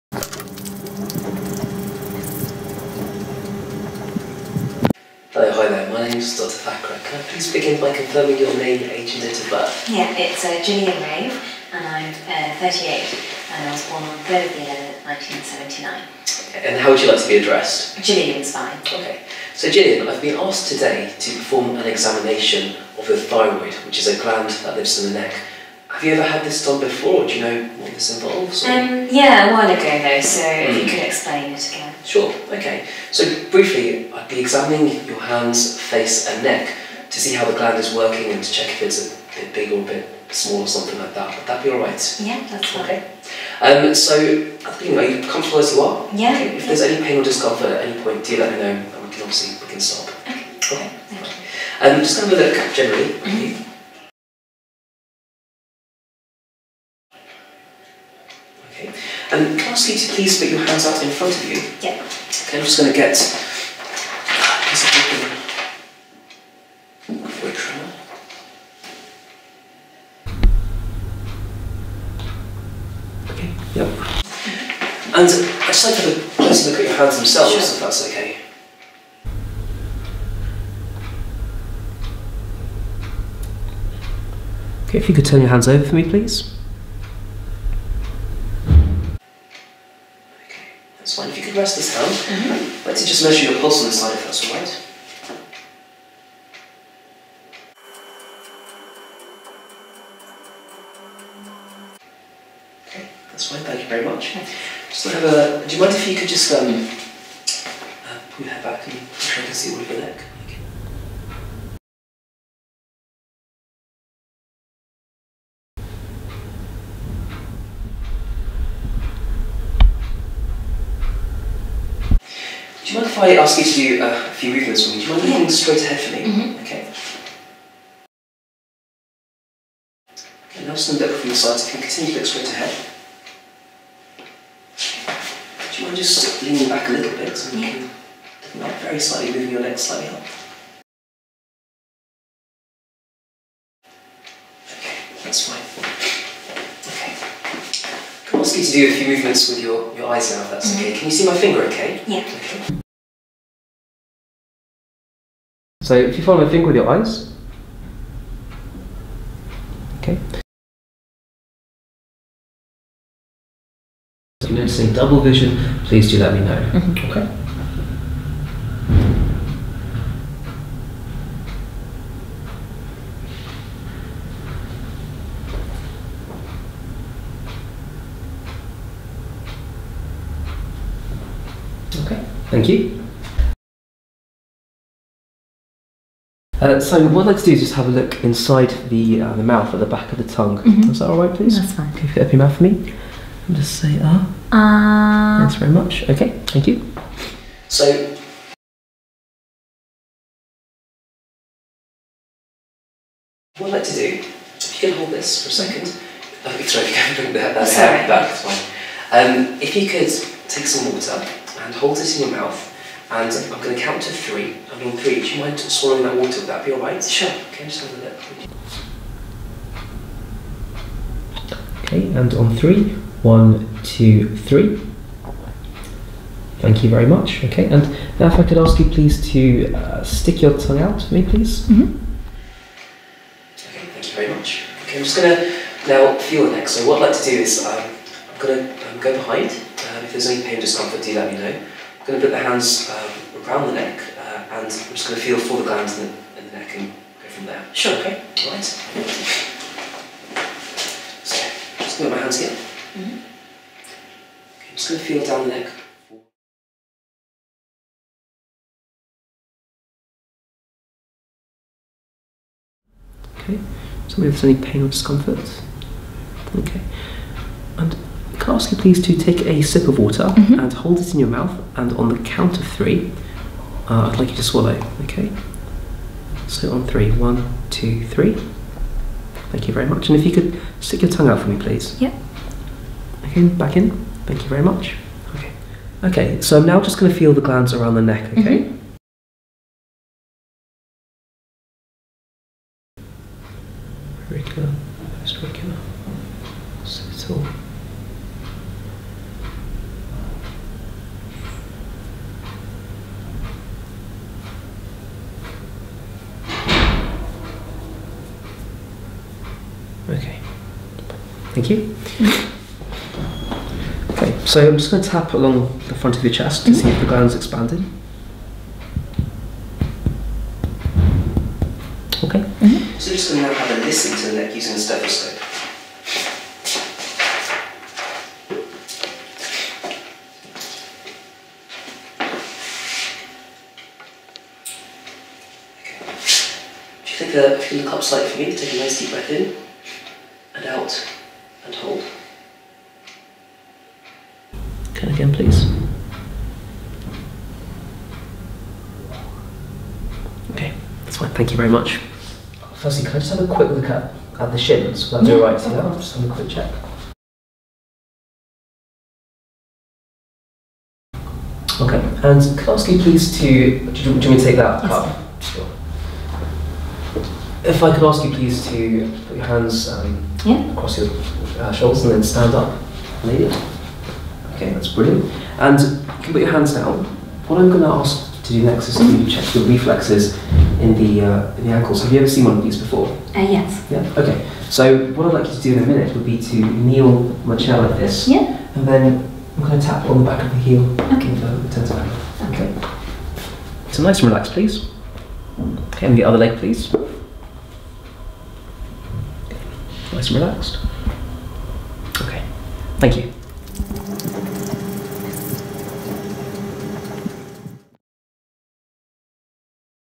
Thyroid Examination
Thyroid examination made for Year 3 students at Hillingdon :)